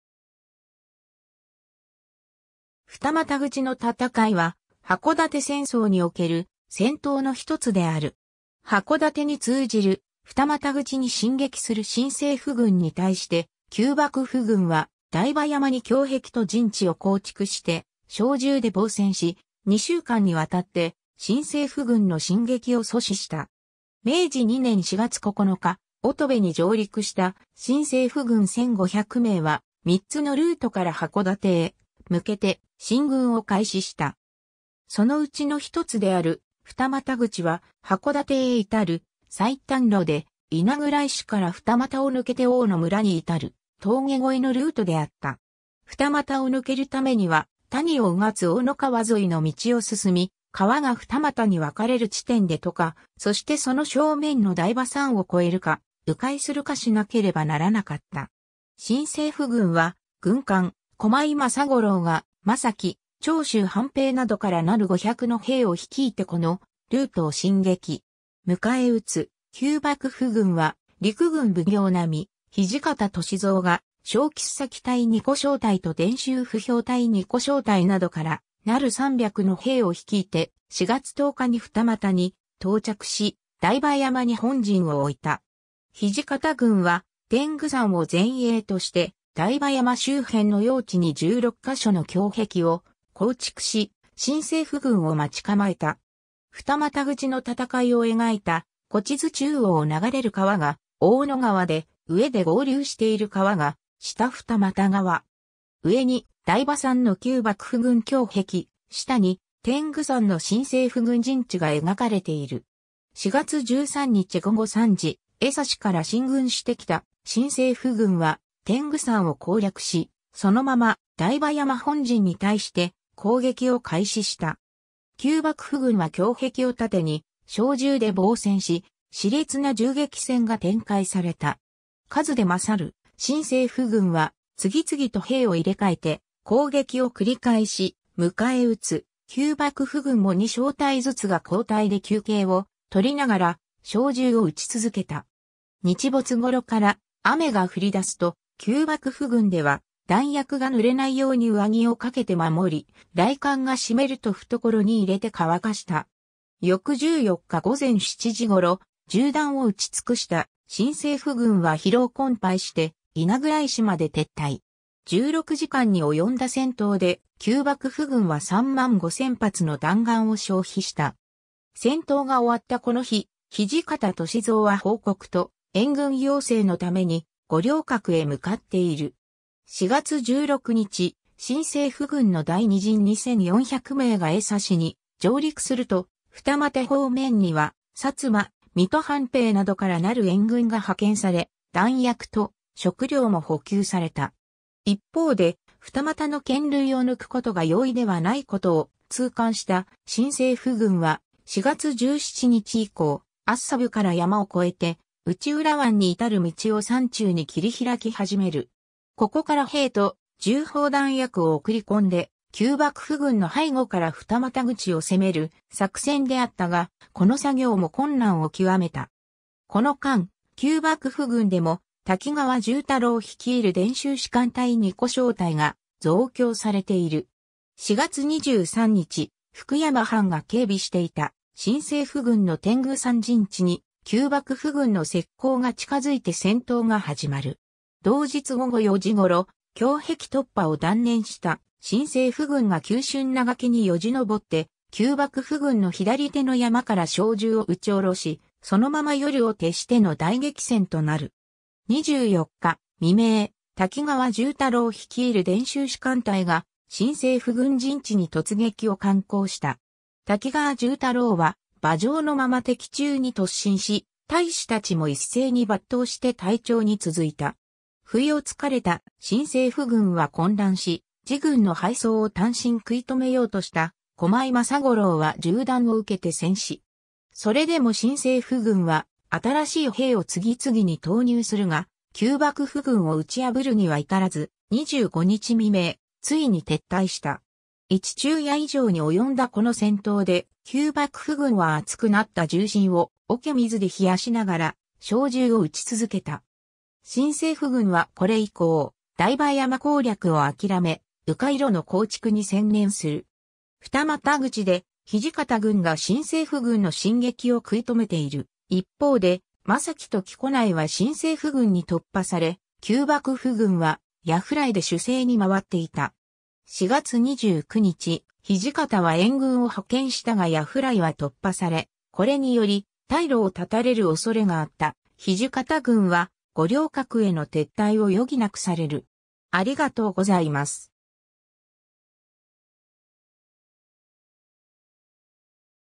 二股口の戦い
二股口の戦い（ふたまたぐちのたたかい、明治2年4月13日 - 4月29日（1869年5月24日 - 6月9日））は、箱館戦争における戦闘のひとつである。
箱館に通じる二股口に進撃する新政府軍に対して、旧幕府軍は台場山に胸壁と陣地を構築して小銃で防戦し、2週間にわたって新政府軍の進撃を阻止した。
 明治2年（1869年）4月9日、乙部に上陸した新政府軍1,500名は、3つのルートから箱館へ向けて進軍を開始した。
そのうちのひとつである二股口は箱館へ至る最短路で、稲倉石から二股を抜けて大野村に至る峠越えのルートであった。
二股を抜けるためには、谷を穿つ大野川沿いの道を進み、川が二又に分かれる地点で渡河、そしてその正面の台場山を越えるか、迂回するかしなければならなかった。
新政府軍は、軍監・駒井政五郎が松前・長州藩兵などからなる500の兵を率いてこのルートを進撃。
迎え撃つ旧幕府軍は、陸軍奉行並・土方歳三が衝鋒隊2個小隊と伝習歩兵隊2個小隊などからなる300の兵を率いて、4月10日に二股に到着し、台場山に本陣を置いた。
土方軍は、天狗山を前衛として台場山周辺の要地に16箇所の胸壁を構築し、新政府軍を待ち構えた。
 二股口の戦いを描いた古地図 中央を流れる川が大野川で、上で合流している川が下二股川。
上に台場山の旧幕府軍胸壁、下に天狗山の新政府軍陣地が描かれている。
 4月13日午後3時、江差から進軍してきた新政府軍は天狗山を攻略し、そのまま台場山本陣に対して攻撃を開始した。
旧幕府軍は胸壁を盾に小銃で防戦し、熾烈な銃撃戦が展開された。
数で勝る新政府軍は、次々と兵を入替えて攻撃を繰り返し、迎え撃つ旧幕府軍も2小隊ずつが交替で休憩をとりながら小銃を撃ち続けた。
日没頃から雨が降り出すと、旧幕府軍では弾薬が濡れないように上着を掛けて守り、雷管が湿ると懐に入れて乾かした。
翌14日午前7時頃、銃弾を撃ちつくした新政府軍は疲労困憊して稲倉石まで撤退。
16時間に及んだ戦闘で、旧幕府軍は3万5000発の弾丸を消費した。
戦闘が終わったこの日、土方歳三は報告と援軍要請のために五稜郭へ向かっている。
 4月16日、新政府軍の第二陣2,400名が江差に上陸すると、二股方面には薩摩・水戸藩兵などからなる援軍が派遣され、弾薬と食糧も補給された。
一方で、二股の堅塁を抜くことが容易ではないことを痛感した新政府軍は、4月17日以降、厚沢部から山を越えて内浦湾に至る道を山中に切り開き始める。
ここから兵と銃砲弾薬を送り込んで、旧幕府軍の背後から二股口を攻める作戦であったが、この作業も困難を極めた。
この間、旧幕府軍でも滝川充太郎率いる伝習士官隊2個小隊が増強されている。
 4月23日、福山藩が警備していた新政府軍の天狗山陣地に旧幕府軍の斥候が近づいて戦闘が始まる。
同日午後4時頃、胸壁突破を断念した新政府軍が急峻な崖によじ登って、旧幕府軍の左手の山から小銃を撃ち下ろし、そのまま夜を徹しての大激戦となる。
24日未明、滝川充太郎率いる伝習士官隊が新政府軍陣地に突撃を敢行した。
滝川充太郎は馬上のまま敵中に突進し、隊士たちも一斉に抜刀して隊長に続いた。
不意を付かれた新政府軍は混乱し、自軍の敗走を単身食い止めようとした駒井政五郎は銃弾を受けて戦死。
それでも新政府軍は新しい兵を次々に投入するが、旧幕府軍を打ち破るには至らず、25日未明、ついに撤退した。
一昼夜以上に及んだこの戦闘で、旧幕府軍は熱くなった銃身を桶水で冷やしながら小銃を撃ち続けた。
新政府軍はこれ以降、台場山攻略をあきらめ、迂回路の構築に専念する。
 二股口で土方軍が新政府軍の進撃を食い止めている一方で、松前と木古内は新政府軍に突破され、旧幕府軍は矢不来で守勢に回っていた。
4月29日、土方は援軍を派遣したが矢不来は突破され、これにより退路を絶たれる恐れがあった土方軍は五稜郭への撤退を余儀なくされる。